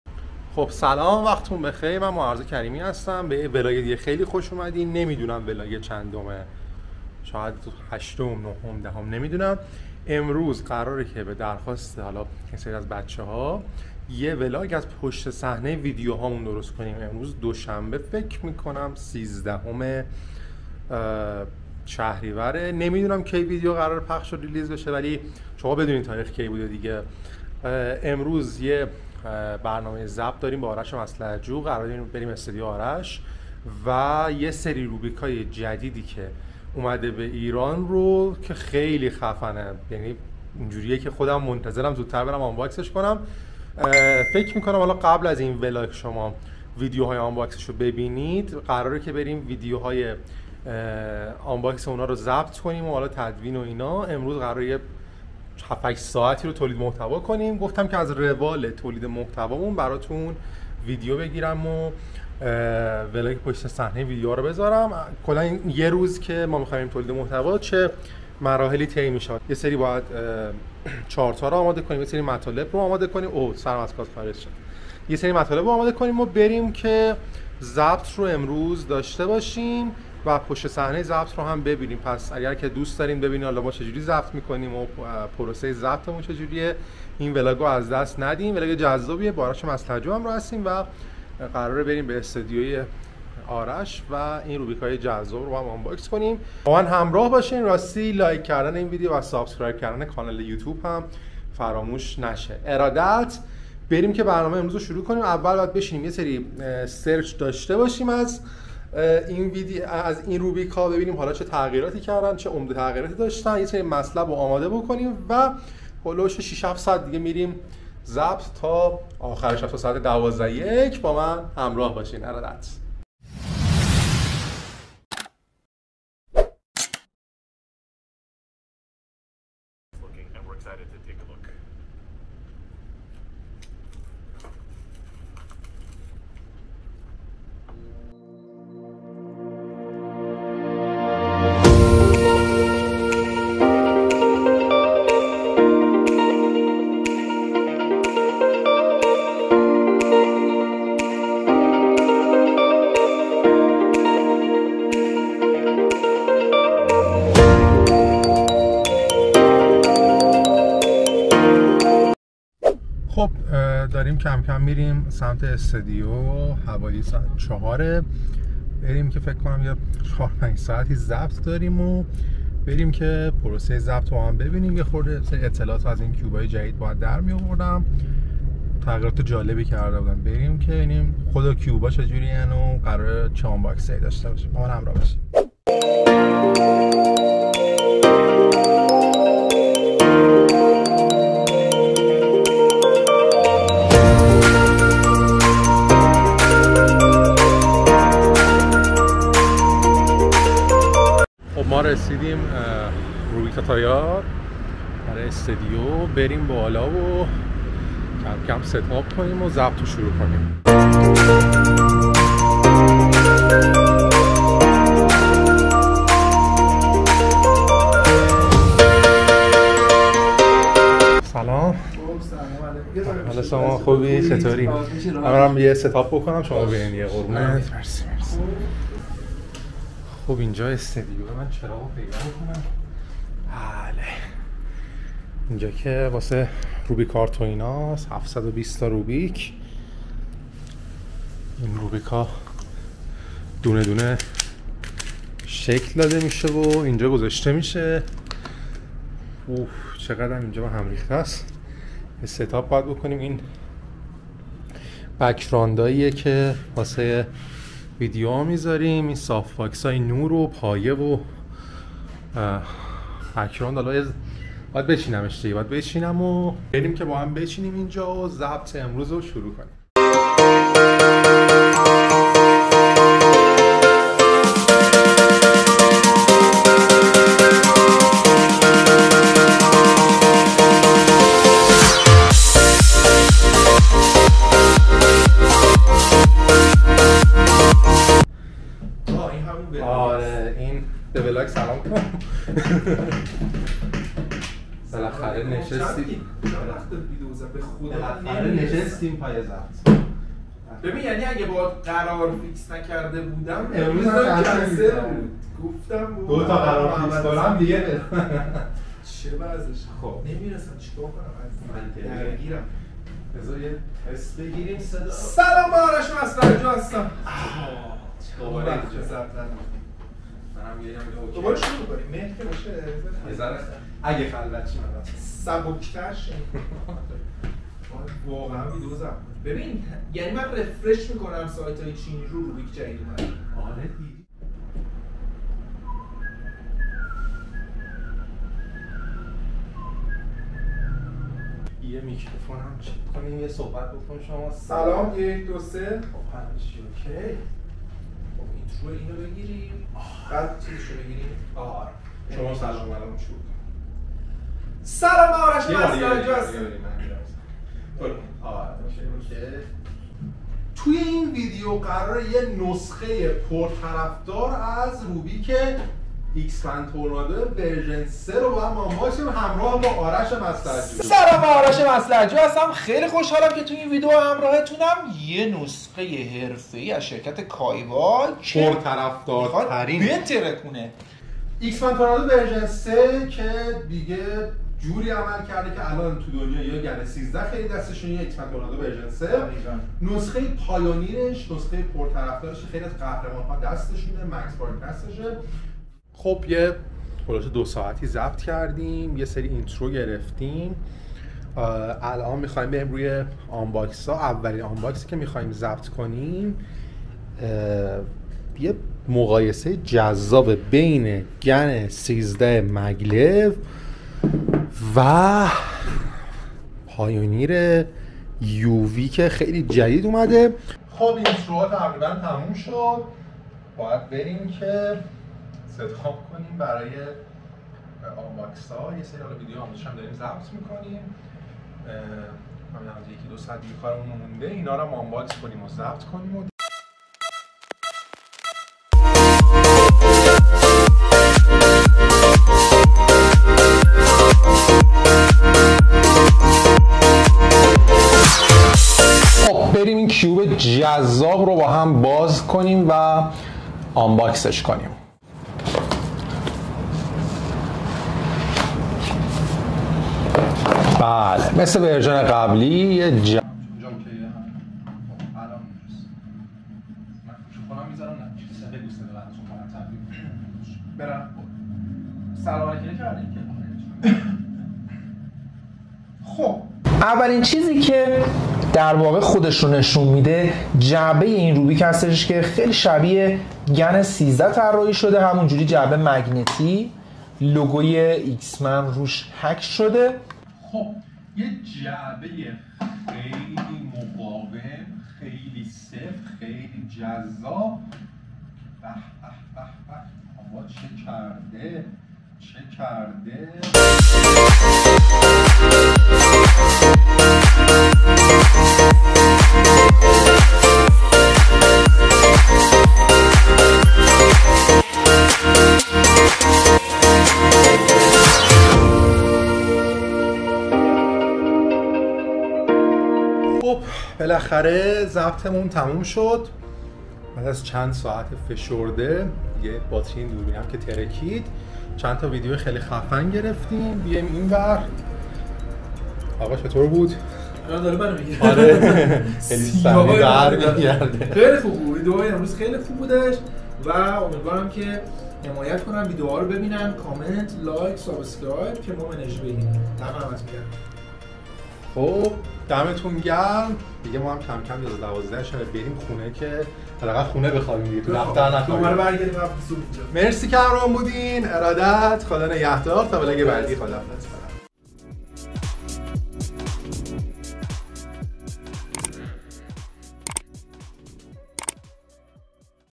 ولاگ پشت صحنه ویدیوهای یوتوب🎬
ولاگ پشت صحنه ضبط ویدیو های بوتیوب🎬تواین ویدیو همراه من باشید تا با مراحل ضبط ویدیوهای من بیشتر آشنا شوید🔥♥️